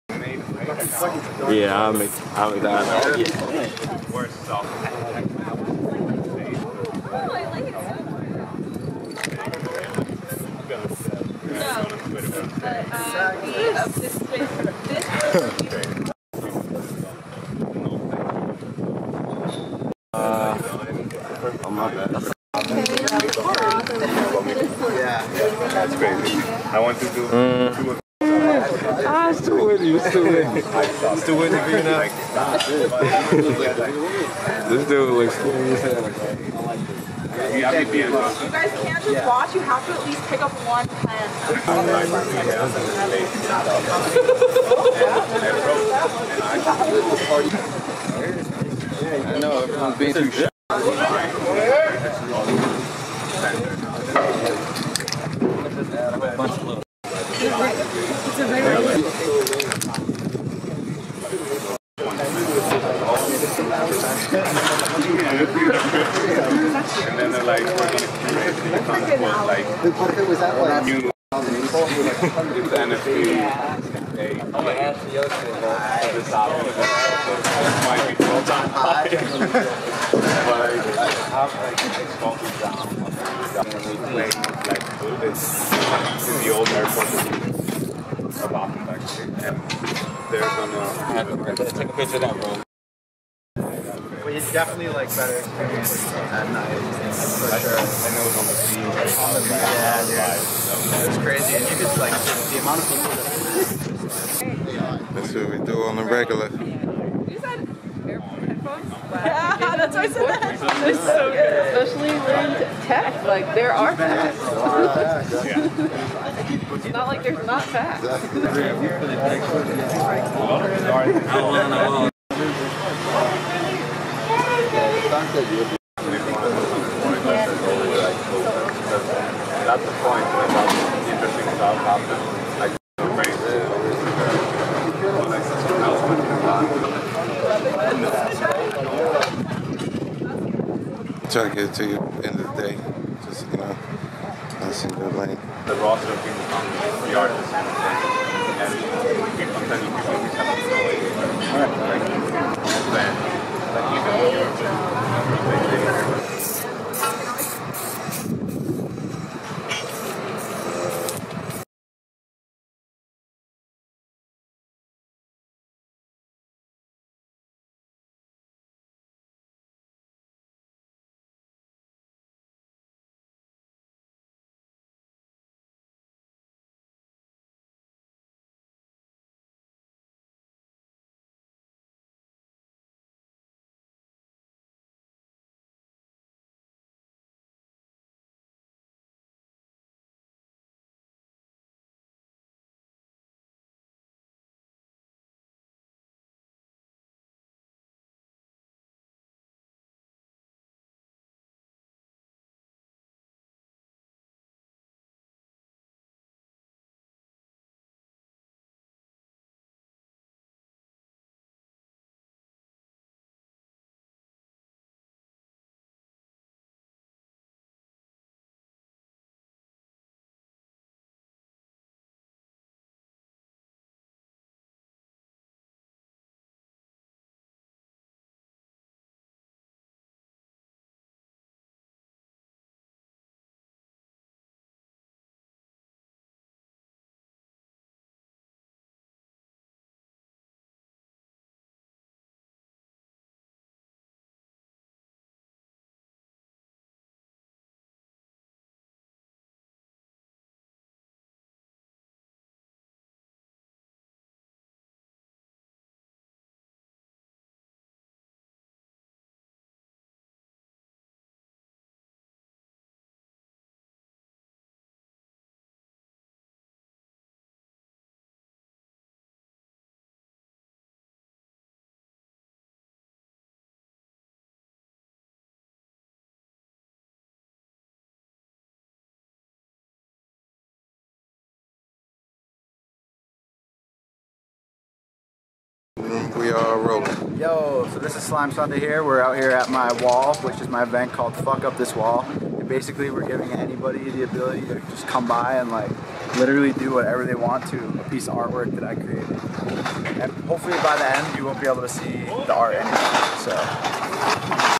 NFT LA collab with Slime Sunday
Goldie, Tuan, and Will linked up with Mike Parisella, better known by the artist's name "Slime Sunday" during NFT LA in March 2022. Slime Sunday is a digital collage artist based out of Salem, MA. His work has been seen in Playboy, Hunger, Plastik, and Glamour magazine. He has been involved with projects with J. Cole, Lana Del Rey, Katy Perry, J Balvin, Beck, and more.

Filmed by Qwon Headen